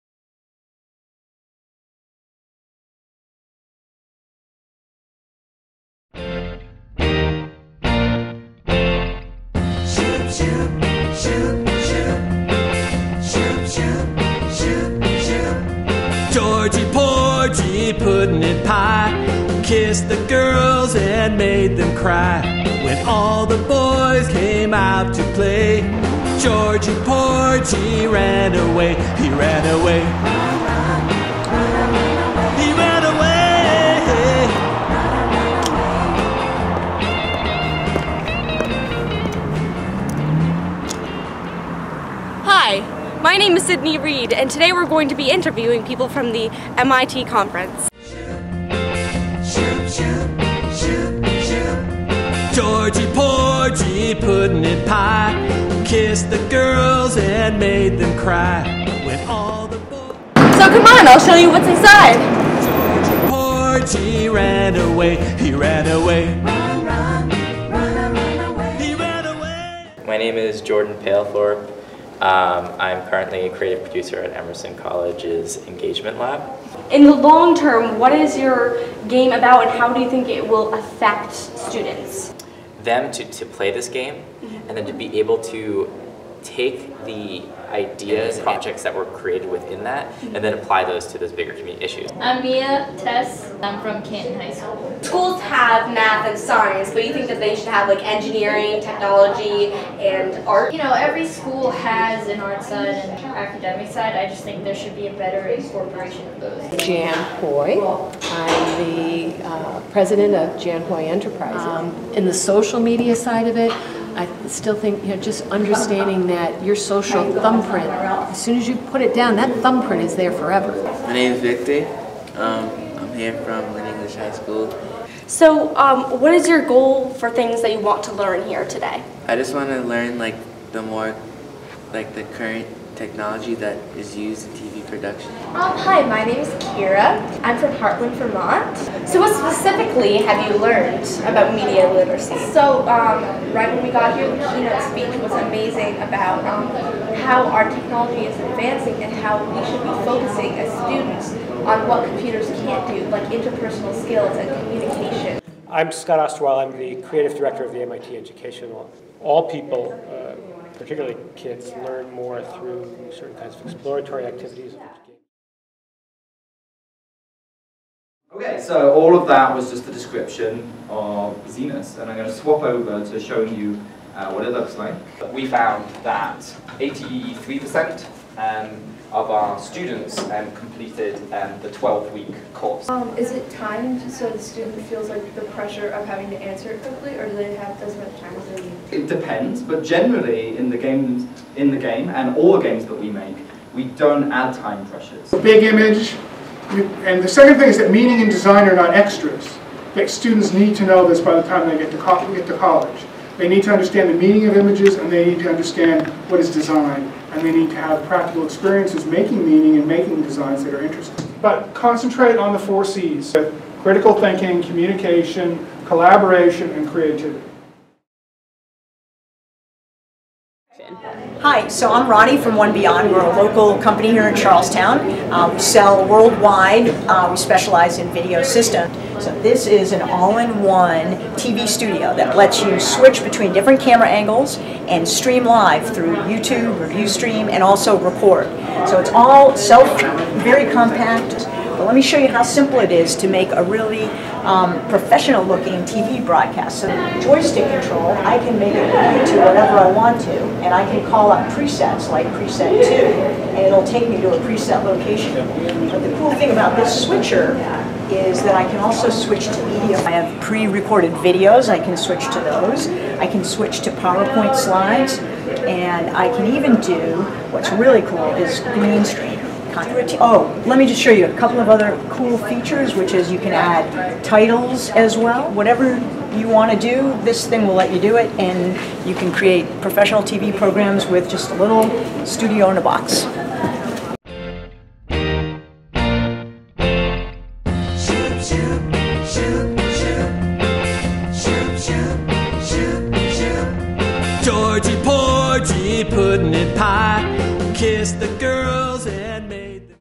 How Media Affects Us - A Discussion at MIT (Media Literacy & The Power of One Conference 2015)
Student TV producers from CATV8, White River Junction produced this video where they interviewed adult and student participants at the conference "Media Literacy & The Power of One."  The conference was held at MIT in November 2015 for high school TV production students, video production teachers, all teachers K-12 and higher education students and instructors.